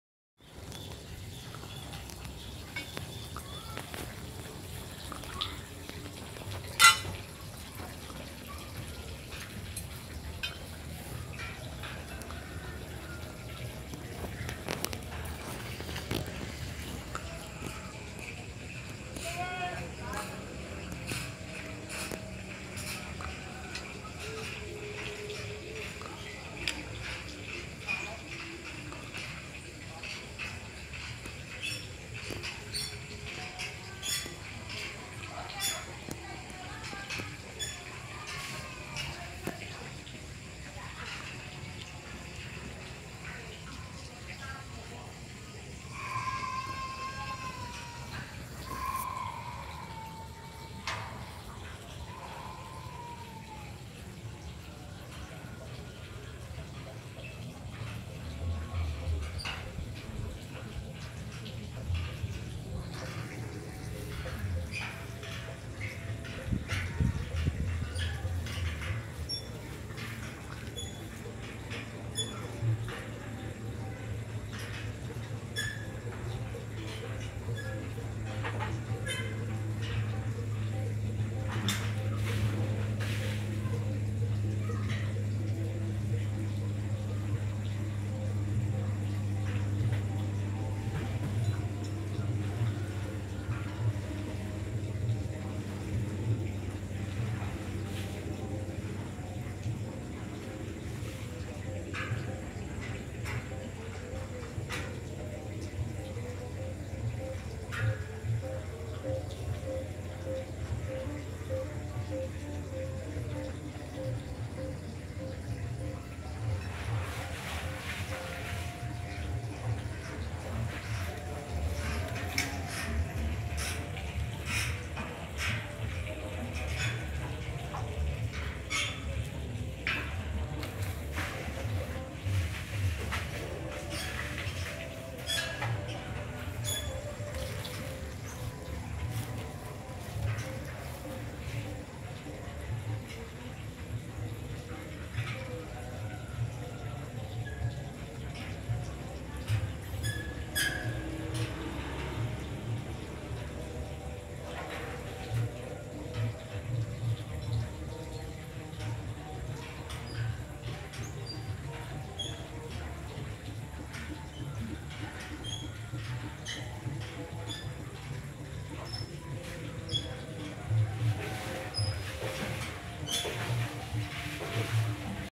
MERINDING! AKSI NEKAT PEKERJA TOWER SUPER TINGGI DI BELAKANG RUMAH
Watching brave men working at very high place